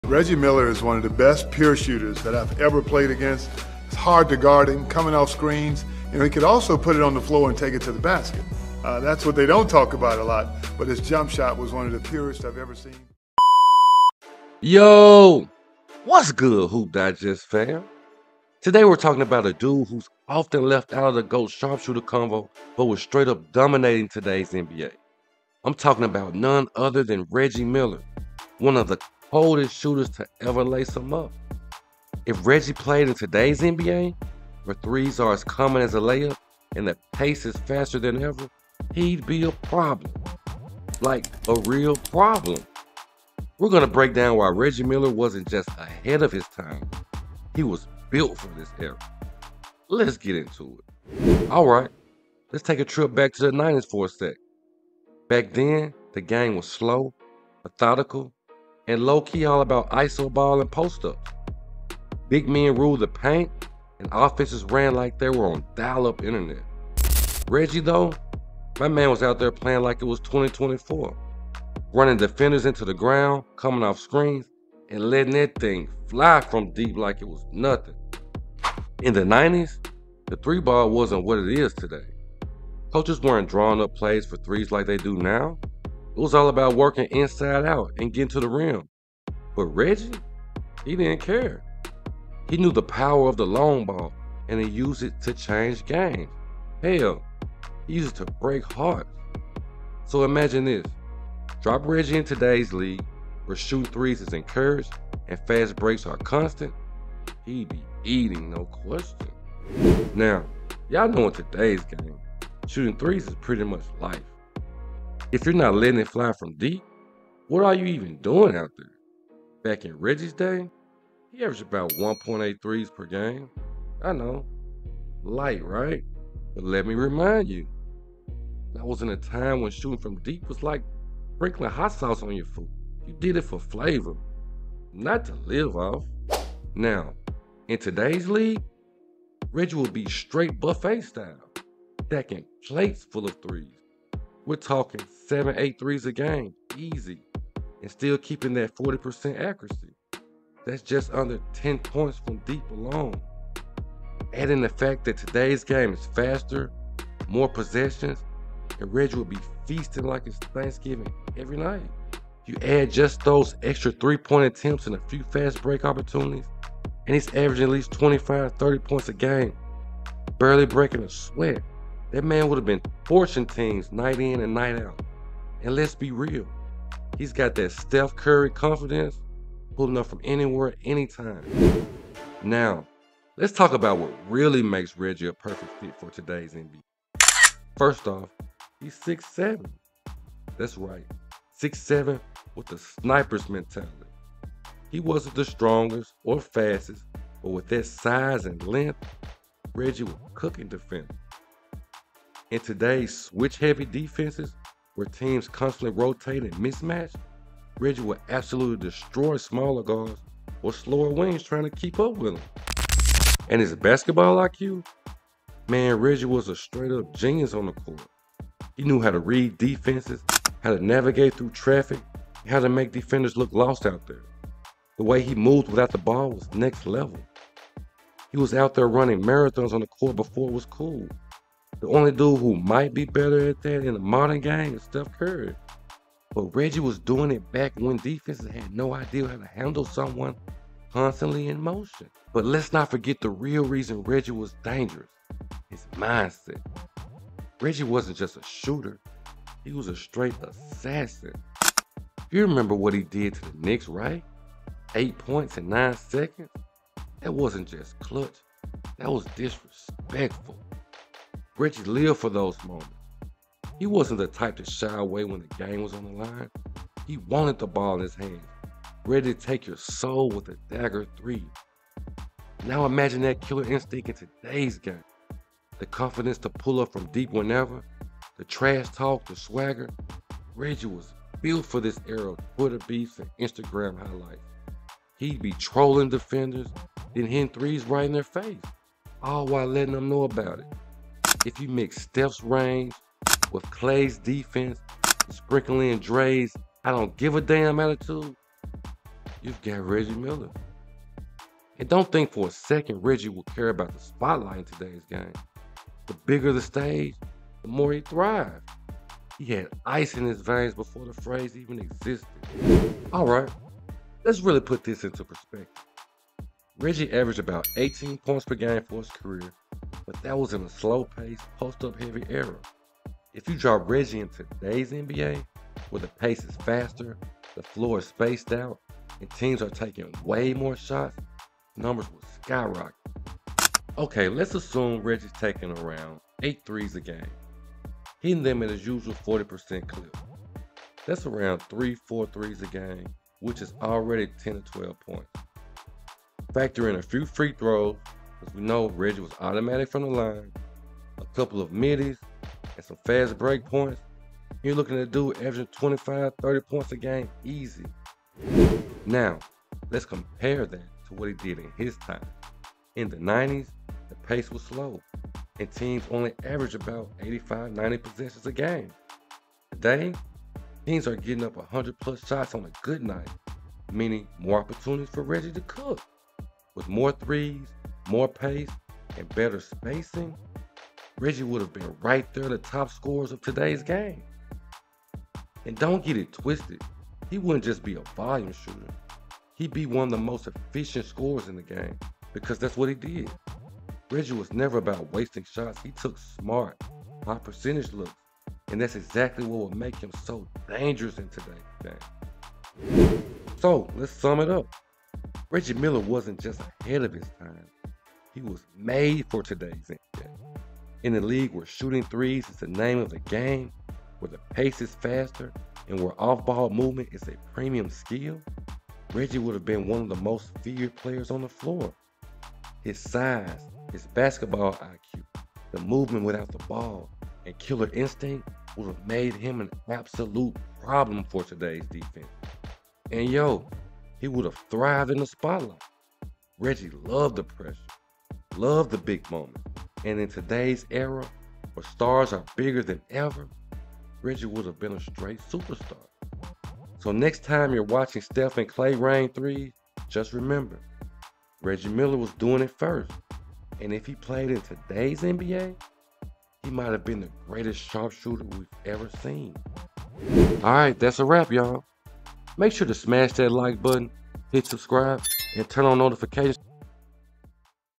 Why Reggie Miller Would DOMINATE TODAY’S NBA!
If you thought Reggie Miller was a force in his era, imagine him in today’s NBA! 🔥 With the 3-point shot ruling the game, Reggie would be smashing records and making headlines. From clutch threes to his legendary trash talk, he was made for this style of play. Steph and Klay might have some serious competition! 👀🏀 Check out why Reggie Miller would be the ultimate shooter in the modern NBA!